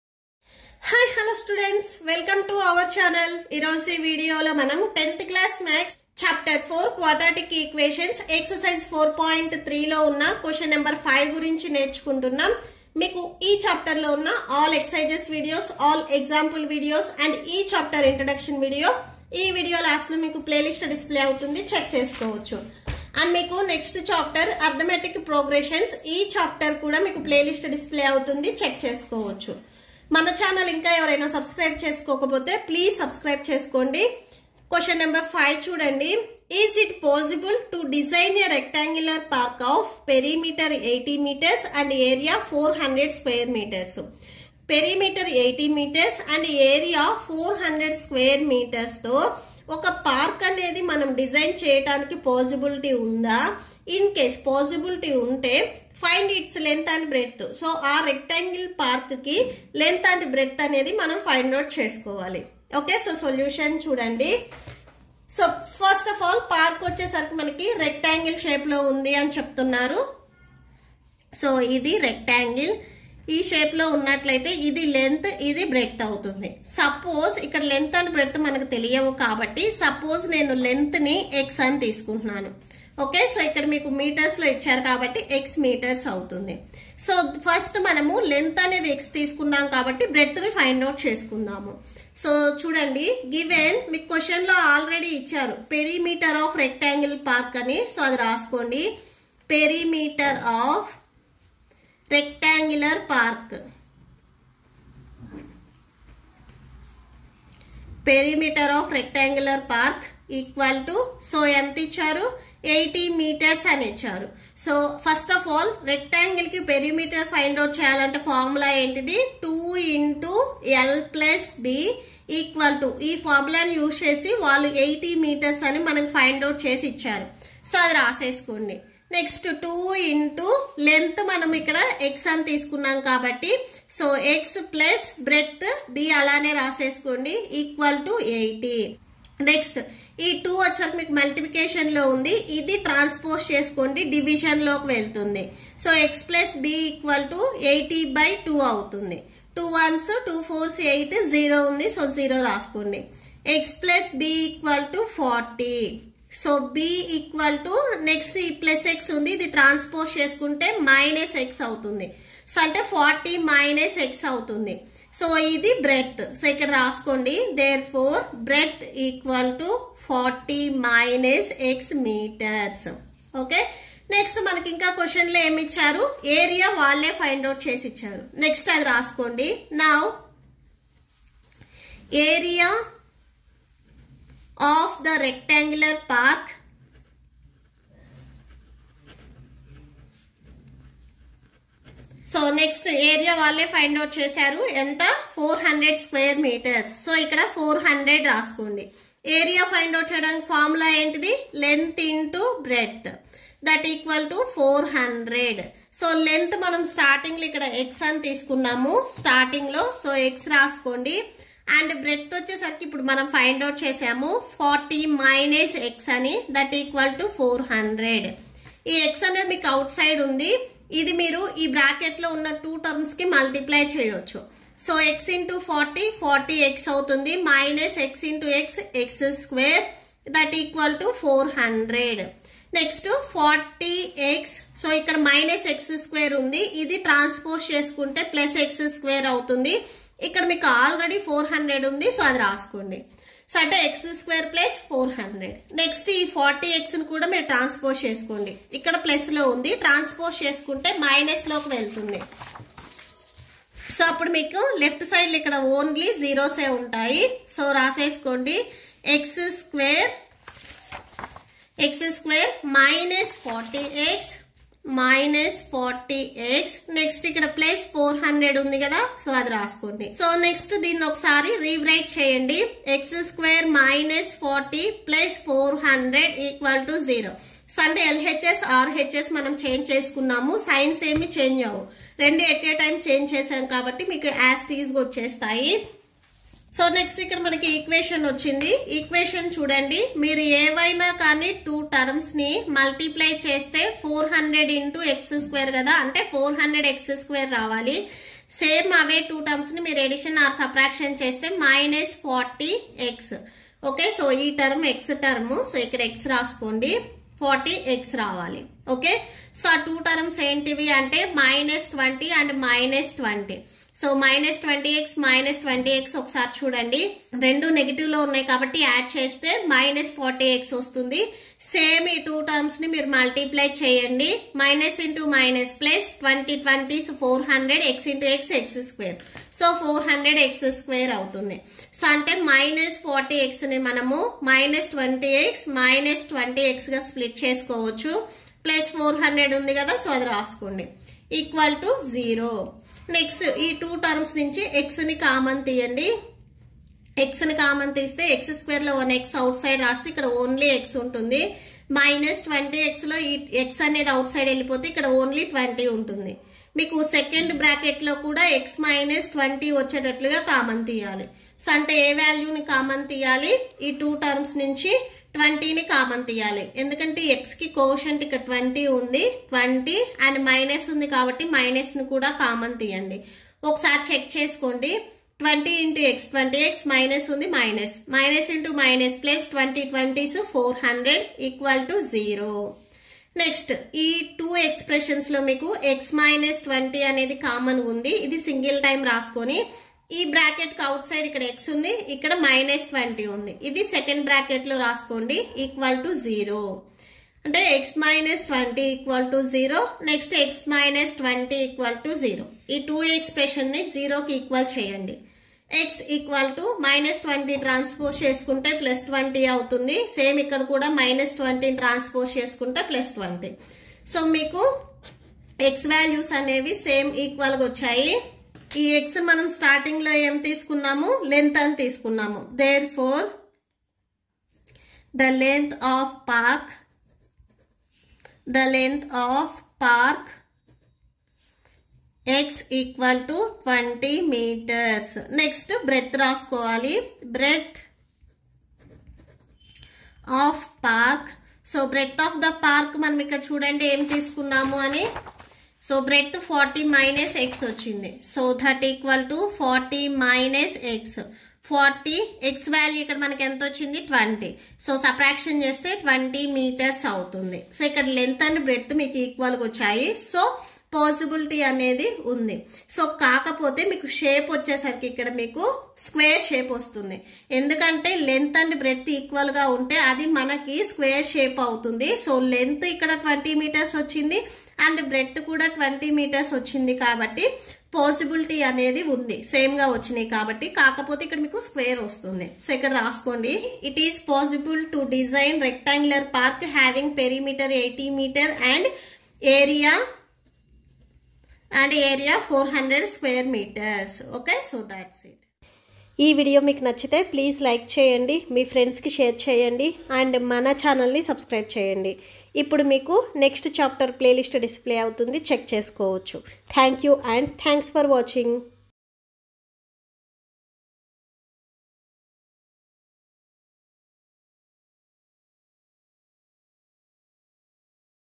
10th class math |chapter 4|Quadratic equations| Exercise 4.3| Q no 5| part 4|CBSE|NCERT|with notes
Exercise 4.3| Q no 5 class10#ex 4.3| Q no 5 class10 10th ex 4.3 Q.no 5 10th ex 4.3 Q.no 5 rectangular park perimeter 80m and area 400m² find its length and breadth#